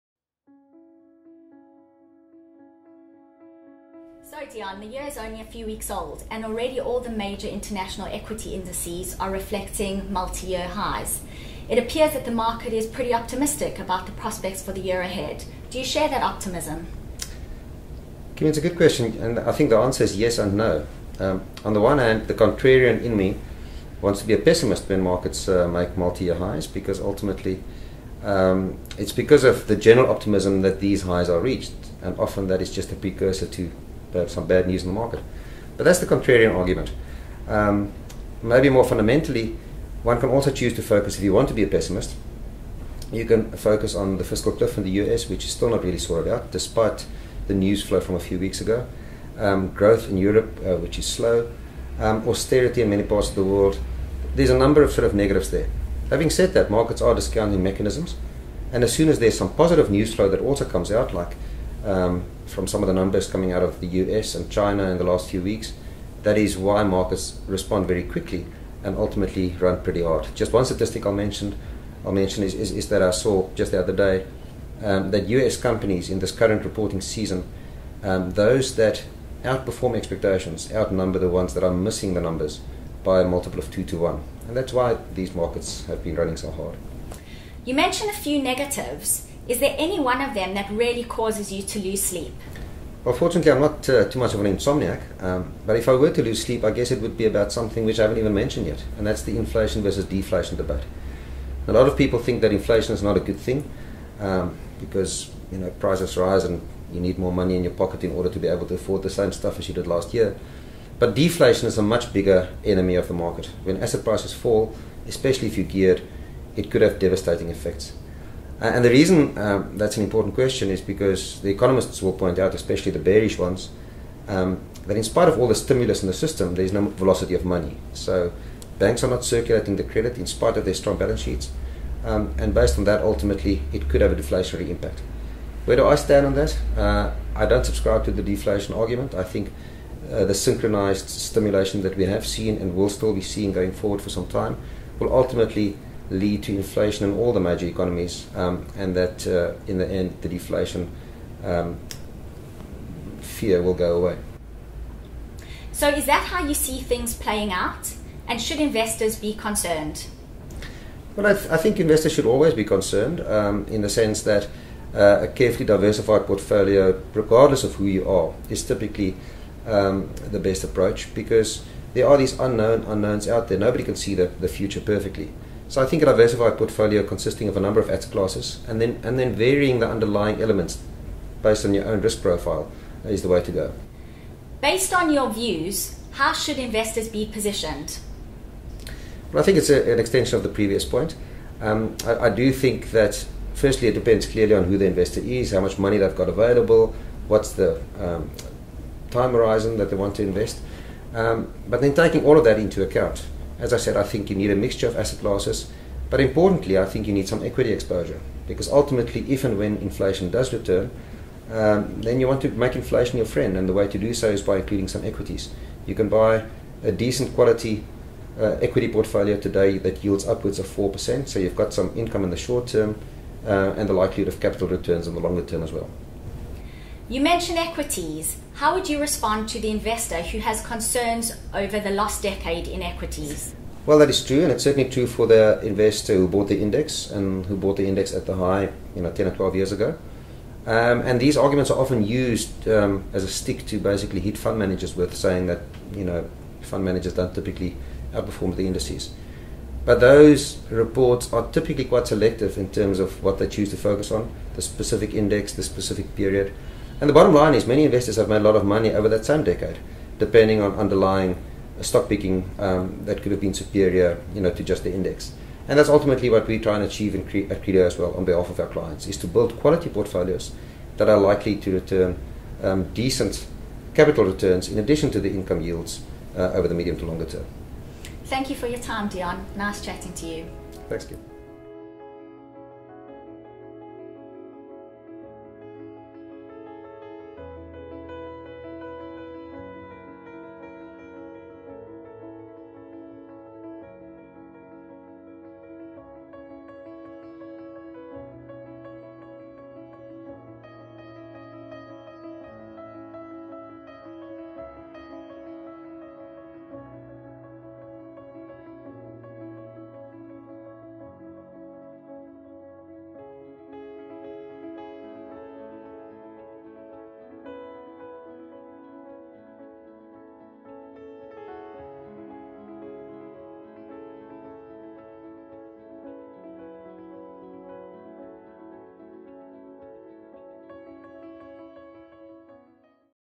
Credo Market Outlook 2013 by Deon Gouws
In this video clip, Chief Investment Officer Deon Gouws shares some market views in conversation with Relationship Manager Kim Simon.  In essence, we acknowledge that financial markets may yet face some very real headwinds, but in spite of that, we remain cautiously optimistic about medium to longer term investment returns.  Accordingly, we believe that clients should retain exposure to e.g. a selection of high quality equities as part of a carefully constructed portfolio.